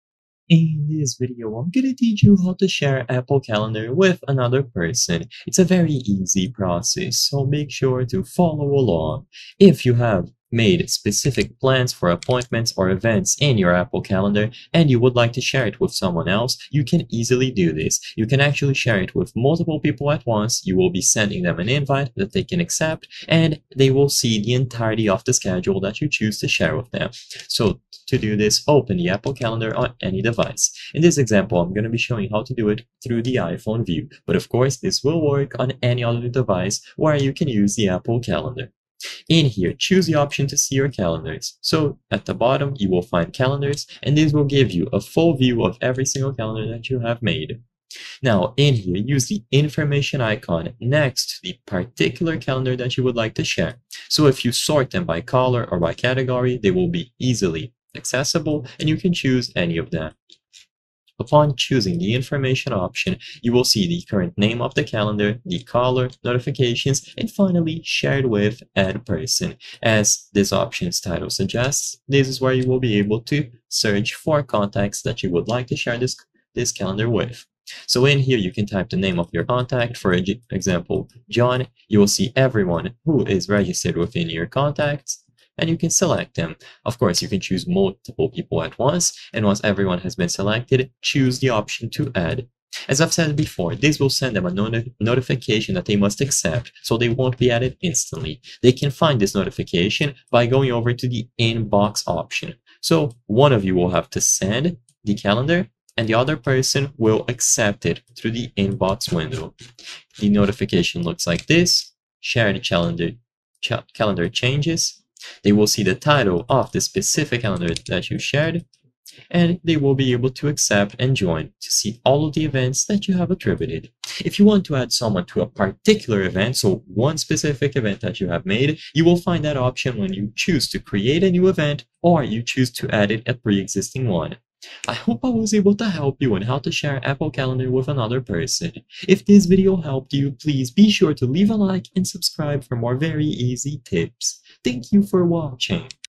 How To Share Apple Calendar With Another Person (2025) - Step by Step
I will solve your doubts about how to share apple calendar with another person, and whether or not it is possible to do this.

I hope I have helped you with the video!